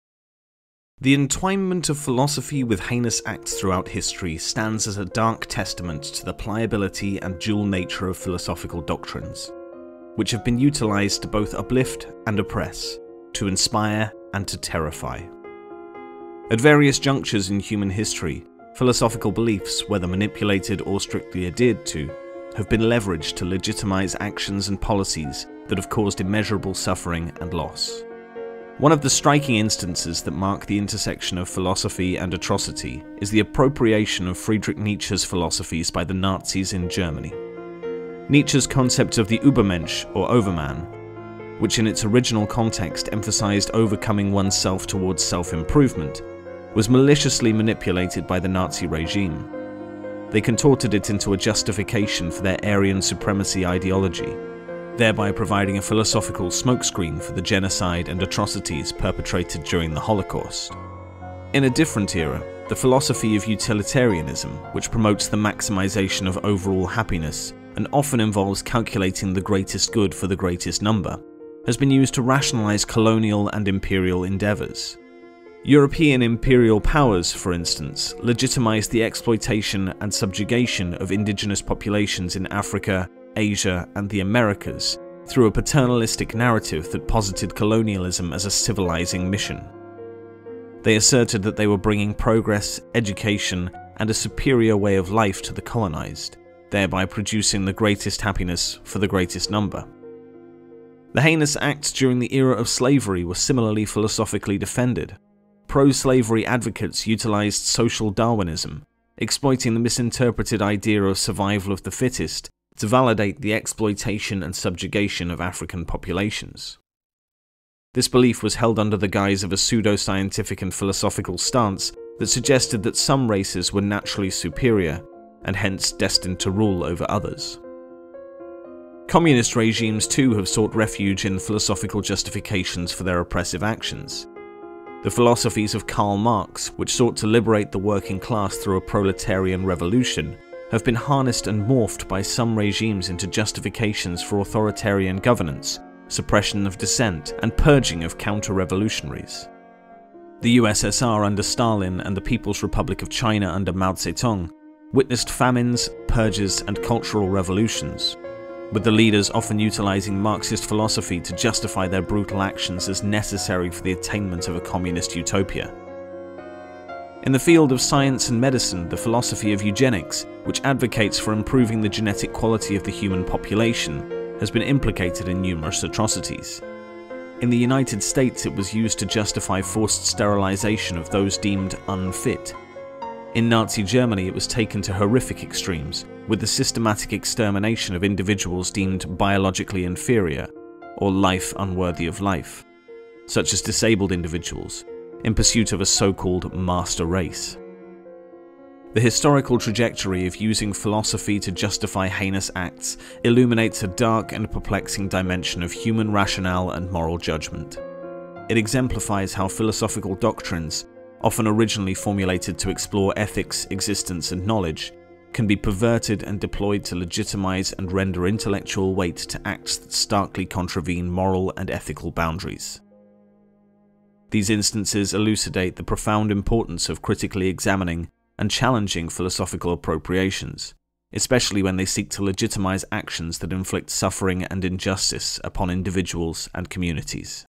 When Philosophy is used to Justify Attrocities
The entwinement of philosophy with heinous acts throughout history stands as a dark testament to the pliability and dual-nature of philosophical doctrines, which have been utilized to both uplift and oppress, to inspire and to terrify. At various junctures in human history, philosophical beliefs, whether manipulated or strictly adhered to, have been leveraged to legitimize actions and policies that have caused immeasurable suffering and loss.

One of the striking instances that mark the intersection of philosophy and atrocity is the appropriation of Friedrich Nietzsche’s philosophies by the Nazis in Germany. Nietzsche's concept of the Übermensch, or “overman”, which in its original context emphasized overcoming oneself towards self-improvement, was maliciously manipulated by the Nazi regime. They contorted it into a justification for their Aryan supremacy ideology, thereby providing a philosophical smokescreen for the genocide and atrocities perpetrated during the Holocaust.

In a different era, the philosophy of Utilitarianism, which promotes the maximization of overall happiness and often involves calculating the greatest good for the greatest number, has been used to rationalize colonial and imperial endeavors. European imperial powers, for instance, legitimized the exploitation and subjugation of indigenous populations in Africa, Asia, and the Americas through a paternalistic narrative that posited colonialism as a ‘civilizing mission.’ They asserted that they were bringing progress, education, and a 'superior' way of life to the colonized, thereby producing the greatest happiness for the greatest number.
The heinous acts during the era of slavery were similarly philosophically defended. Pro-slavery advocates utilized Social Darwinism, exploiting the misinterpreted idea of "survival of the fittest" to validate the exploitation and subjugation of African populations. This belief was held under the guise of a pseudo-scientific and philosophical stance that suggested that some races were naturally superior and hence, destined to rule over others.

Communist regimes too have sought refuge in philosophical justifications for their oppressive actions. The philosophies of Karl Marx, which sought to liberate the working class through a proletarian revolution, have been harnessed and morphed by some regimes into justifications for authoritarian governance, suppression of dissent, and purging of ‘counter-revolutionaries’. The USSR under Stalin and the People’s Republic of China under Mao Zedong witnessed famines, purges, and cultural revolutions, with the leaders often utilizing Marxist philosophy to justify their brutal actions as necessary for the attainment of a communist utopia.
In the field of science and medicine, the philosophy of eugenics, which advocates for improving the genetic quality of the human population, has been implicated in numerous atrocities. In the United States, it was used to justify forced sterilizations of those deemed 'unfit'. In Nazi Germany, it was taken to horrific extremes with the systematic extermination of individuals deemed ‘biologically inferior’ or ‘life unworthy of life’, such as disabled individuals, in pursuit of a so-called ‘master race’.

The historical trajectory of using philosophy to justify heinous acts illuminates a dark and perplexing dimension of human rationale and moral judgement. It exemplifies how philosophical doctrines, often originally formulated to explore ethics, existence, and knowledge, can be perverted and deployed to legitimize and render intellectual weight to acts that starkly contravene moral and ethical boundaries. These instances elucidate the profound importance of critically examining and challenging philosophical appropriations, especially when they seek to legitimize actions that inflict suffering and injustice upon individuals and communities.